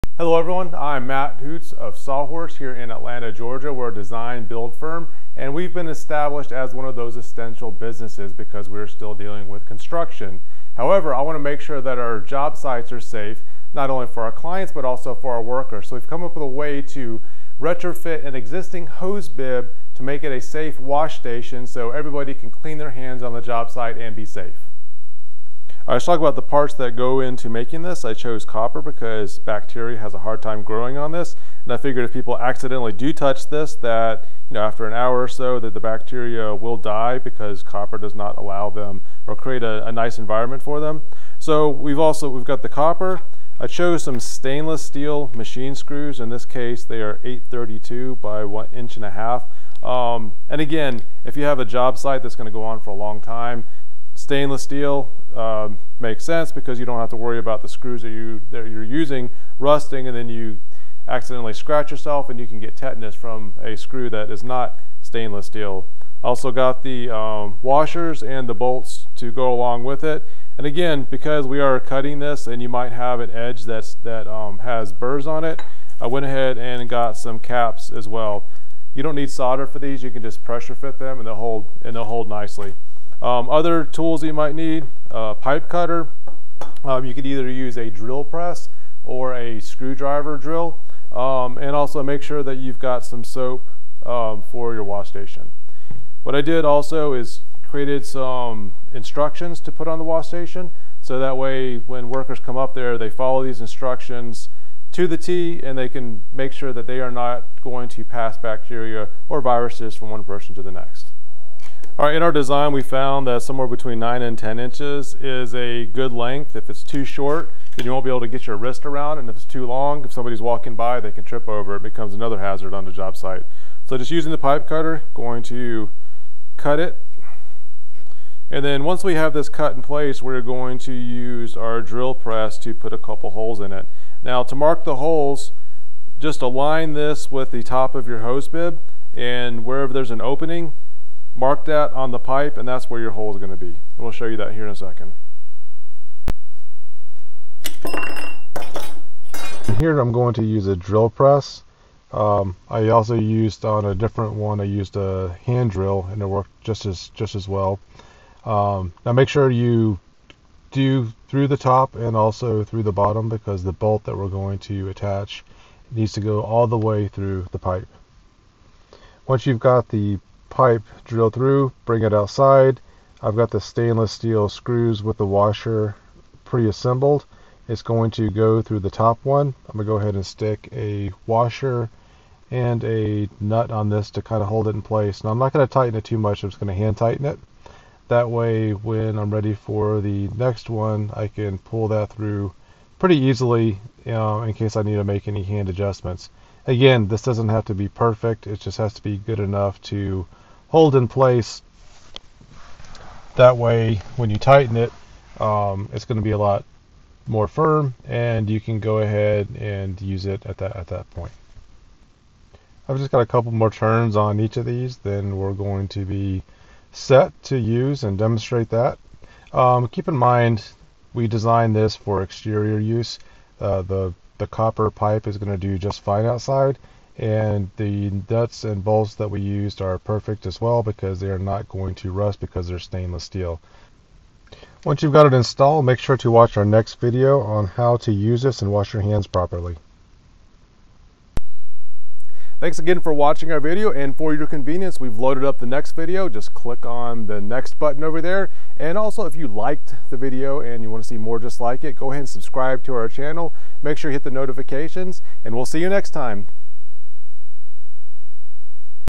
How to make a DIY COVID19 Construction Jobsite Hand-Washing Station
Construction is still listed as an "essential business" and we have a responsibility for the safety of our workers and clients on the job site.

Most construction job-sites are poorly equipped for cleaning hands in accordance with the CDC.  We created a hands free device that can attach to any external hose bibb to allow proper hand cleaning on the job site.

This video shows how to design, fabricate and attach the "Hands Free" Jobsite Wash Station.

List of materials used:
1/2 Copper Pipe
1/2 Copper End Cap
8-32 x 1.5" Stainless Steel Machine Screws
Stainless Steel Washer (4) and nuts (2)

Tools used:
Pipe Cutter
Drill Press or Hand Drill

Chapters:
Intro 0:00
What you will need 0:40
Measurements and cuts 2:06
Installing the station 3:06